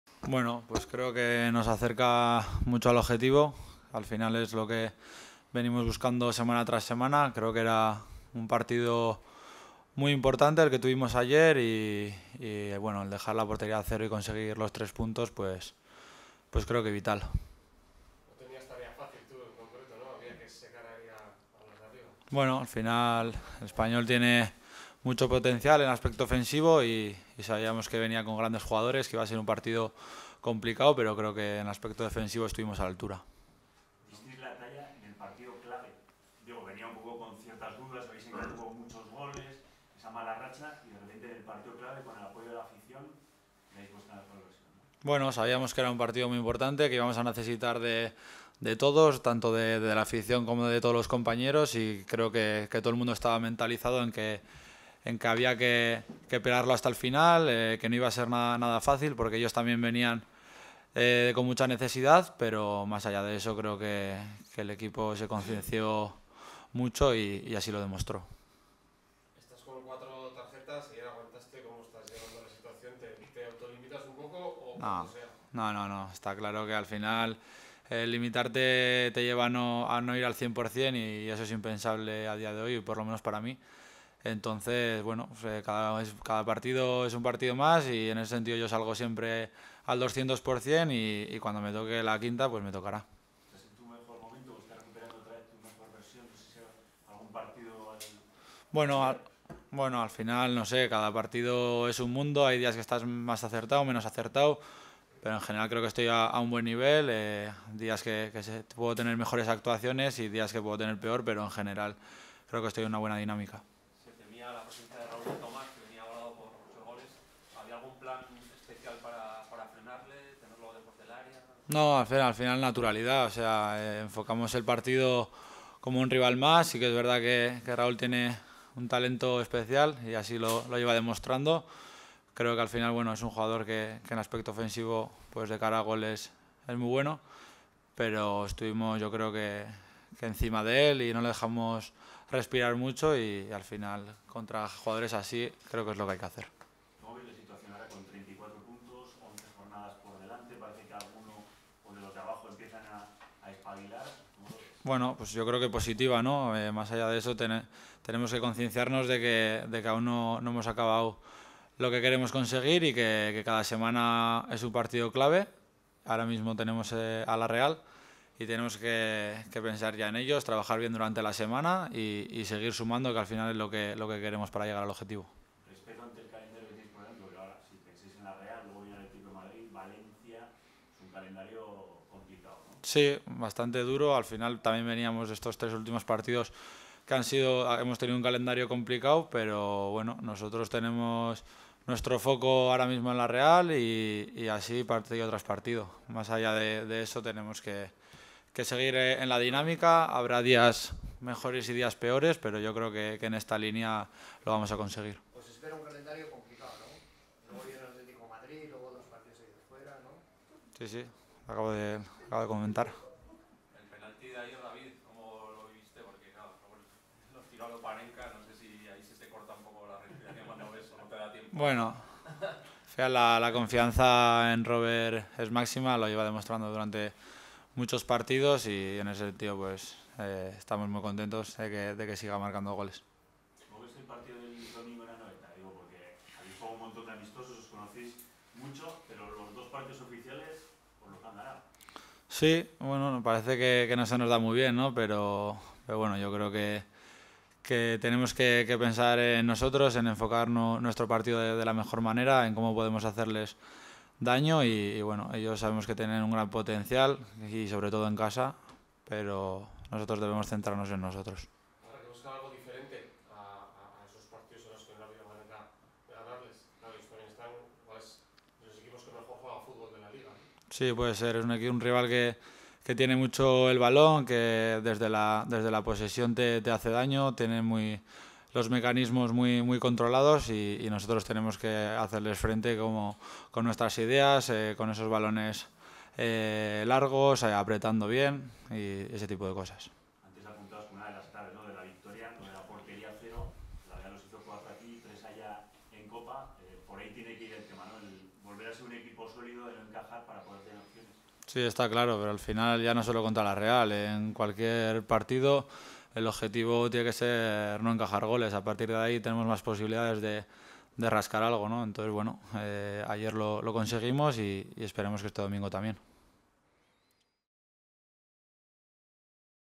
David García: "La victoria nos acerca al objetivo"
Rueda de prensa de David García en las instalaciones de Tajonar.